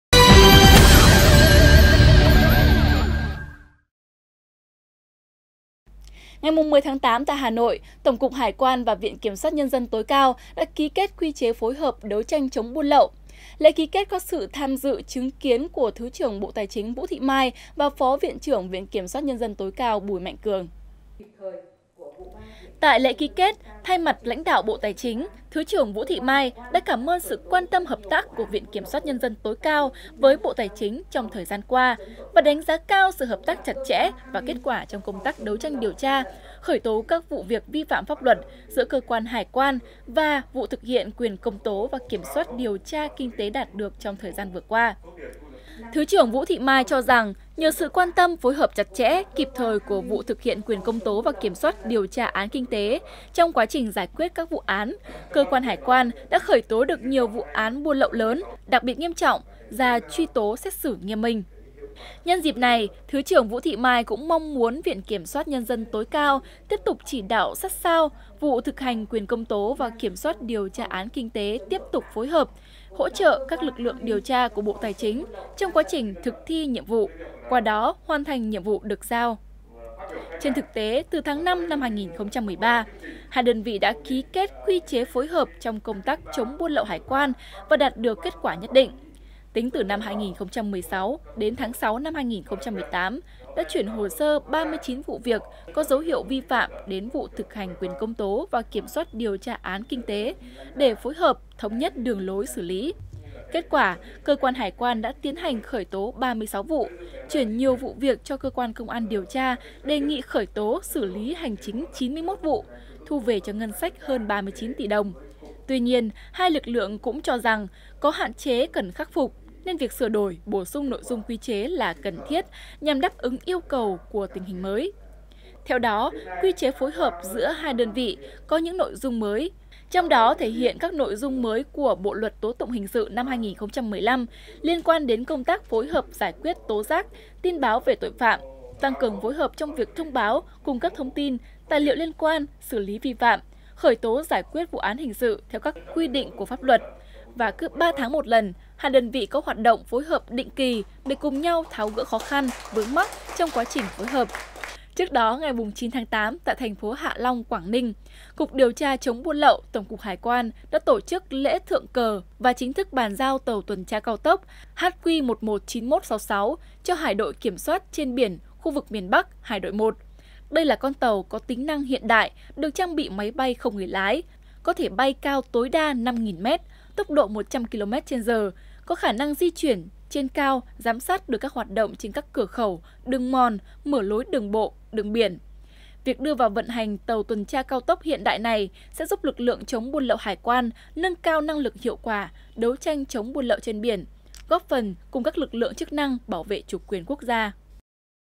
Tổng cục Hải quan và Viện Kiểm soát nhân dân tối cao tăng cường phối hợp đấu tranh chống buôn lậu
Ngày 10/8, tại Hà Nội, Tổng cục Hải quan và Viện Kiểm soát nhân dân tối cao đã ký kết quy chế phối hợp đấu tranh chống buôn lậu. Lễ ký kết có sự tham gia chứng kiến của Thứ trưởng Bộ Tài chính Vũ Thị Mai và Phó Viện trưởng Viện Kiểm soát nhân dân tối cao Bùi Mạnh Cường.